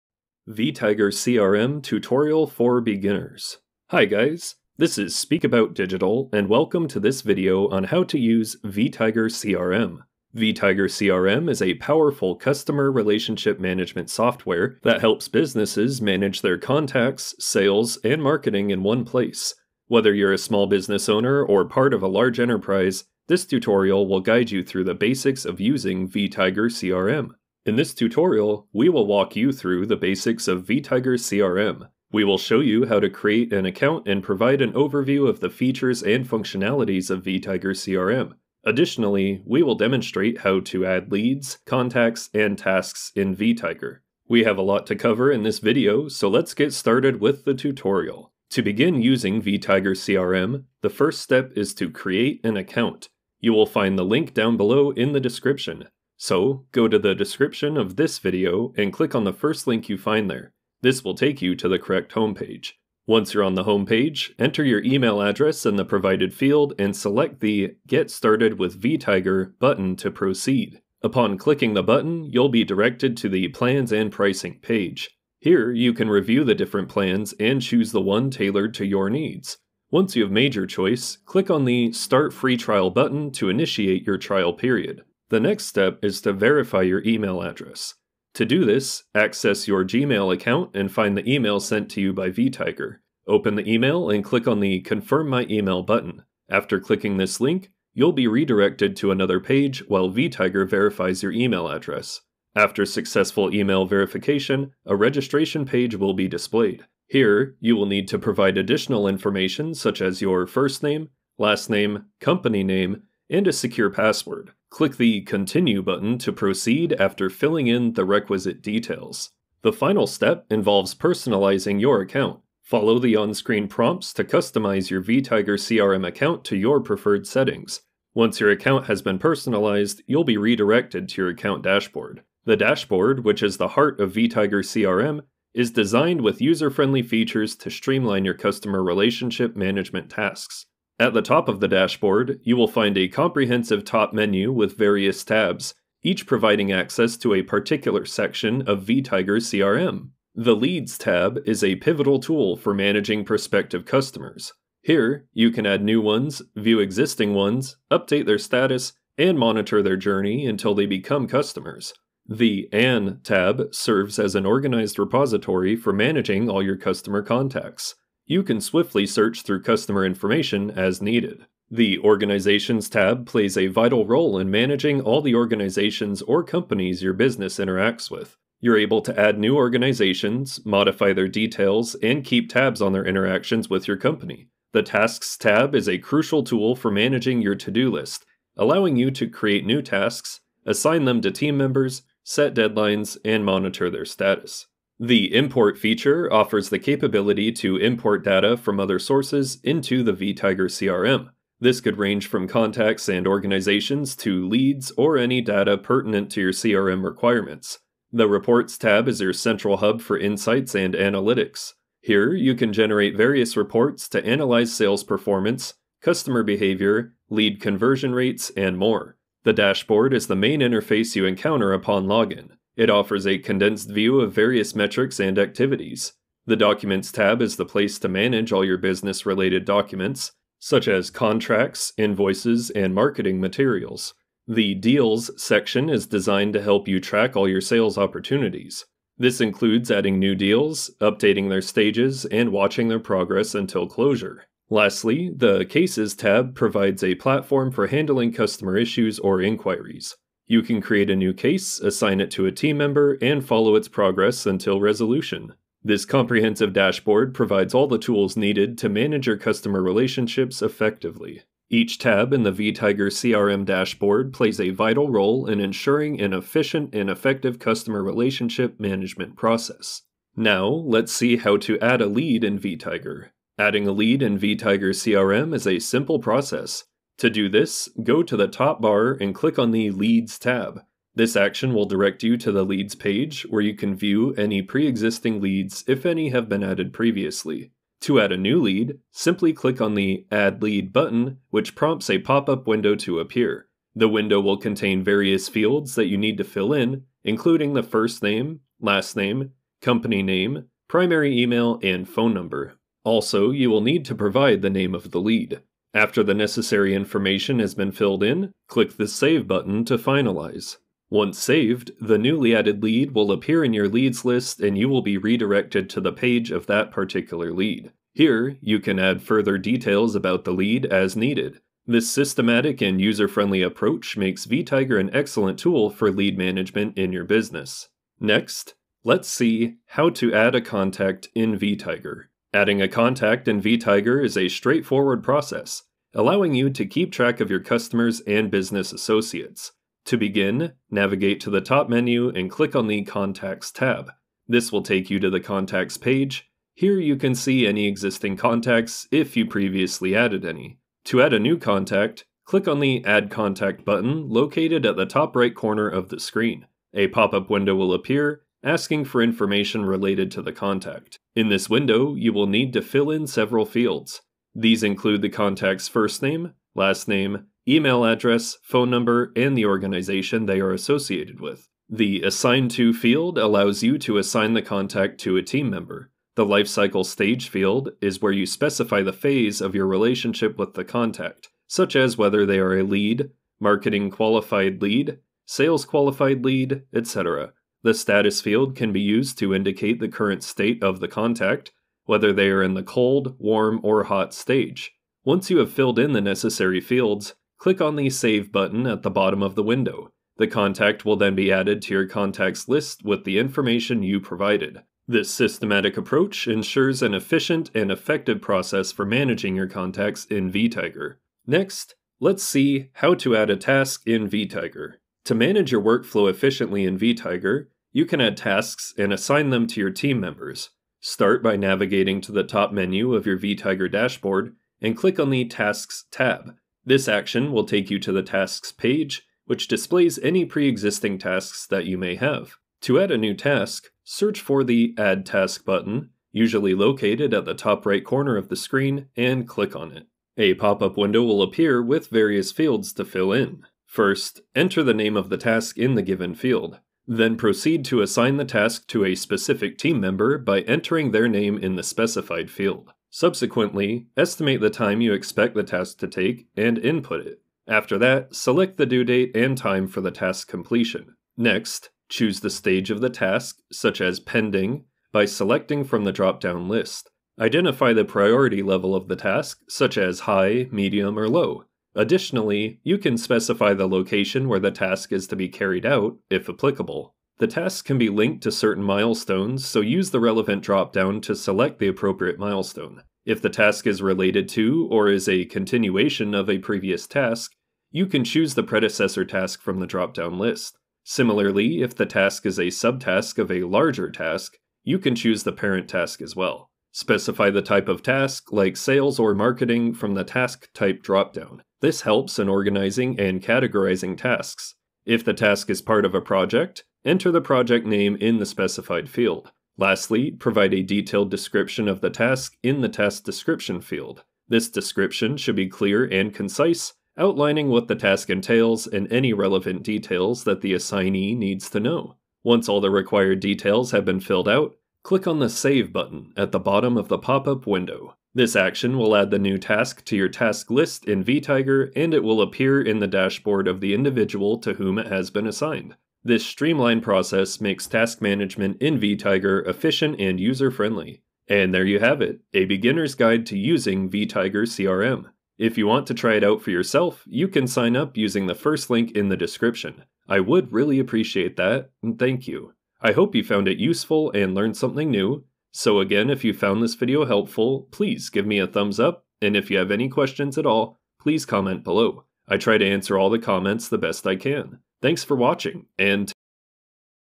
Vtiger CRM Tutorial For Beginners | How To Use Vtiger CRM
This is my video tutorial on Vtiger CRM for beginners. I cover the most basic functions.

How To Use Vtiger CRM - Vtiger CRM Tutorial For Beginners